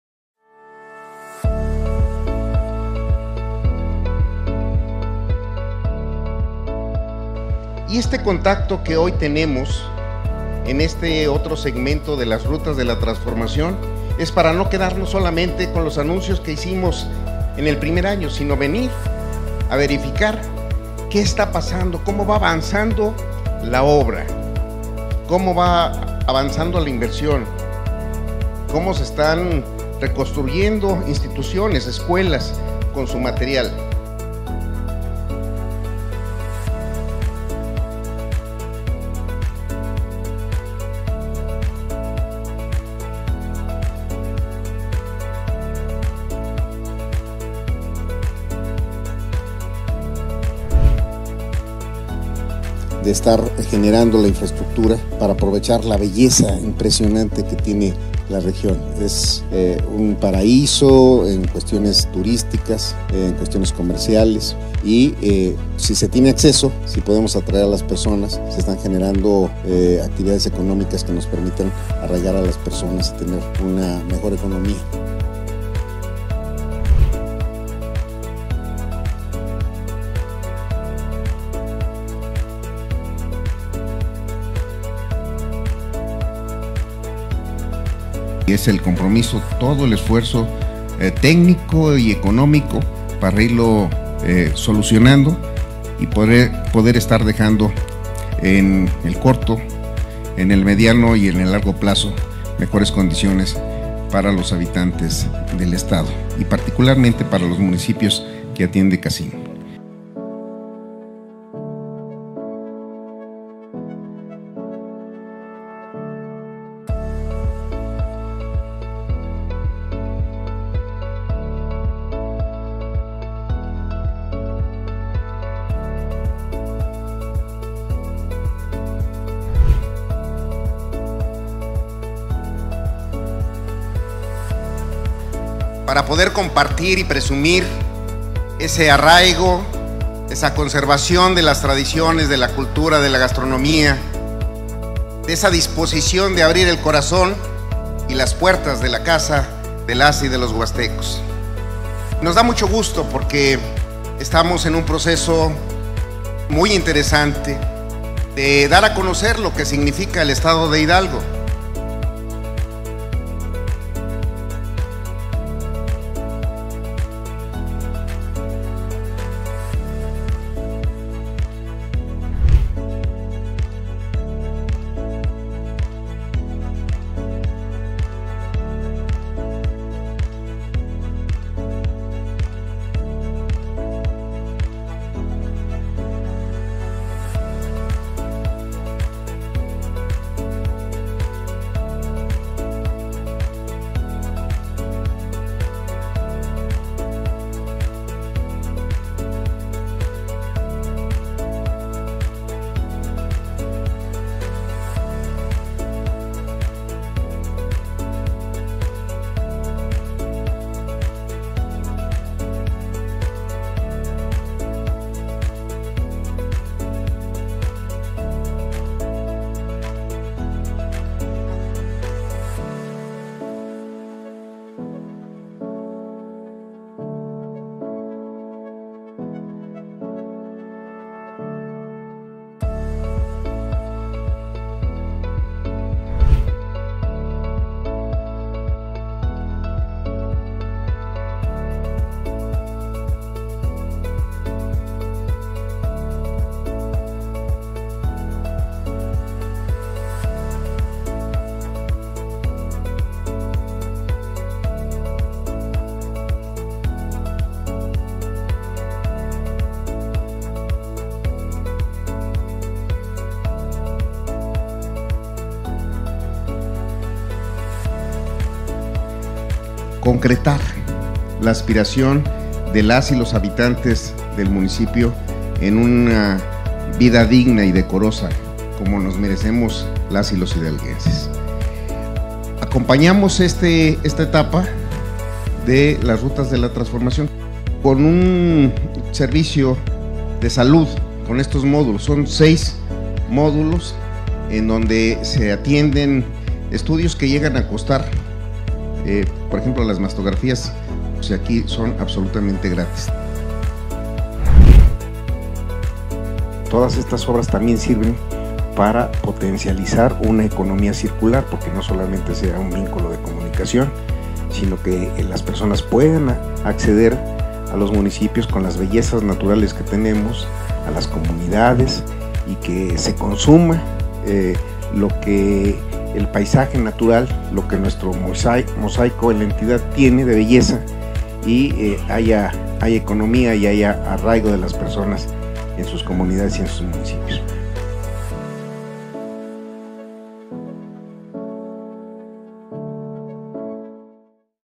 Resumen de actividades 13 al 19 de noviembre | Julio Menchaca
Les comparto el trabajo que realizamos la semana pasada, donde con cercanía y acciones concretas seguimos transformando a Hidalgo.